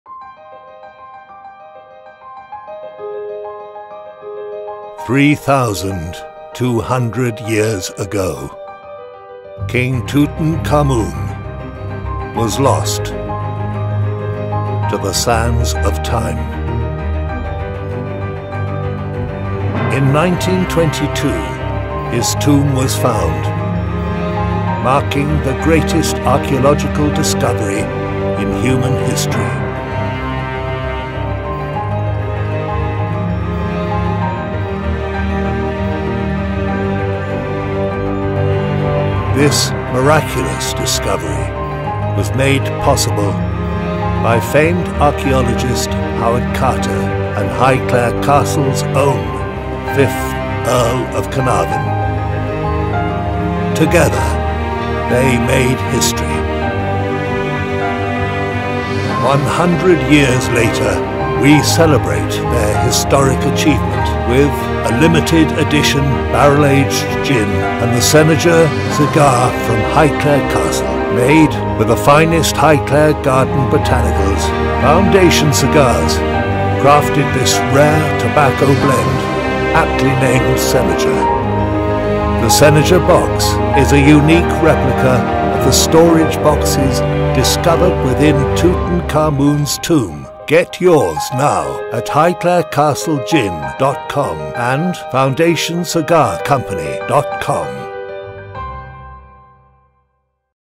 Highclere Castle Barrel Aged Gin & The Highclere Castle Senetjer Cigar
100 years ago this year, Lord Carnarvon left Highclere Castle for the sands of Egypt and discovered the tomb of Tutankhamen.  To commemorate this historic event, we have crafted two rare and special products.